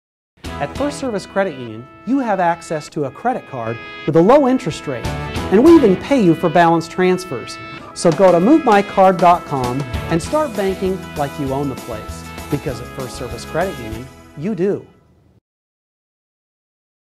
First Service Credit Union - Low Interest Credit Cards
It's Time to Get the Credit YOU Deserve! The First Service Credit Union Platinum MasterCard includes low rates, no annual fee, no balance transfer fee, and 1% cash back on balance transfers. Visit MoveMyCard.com to apply today!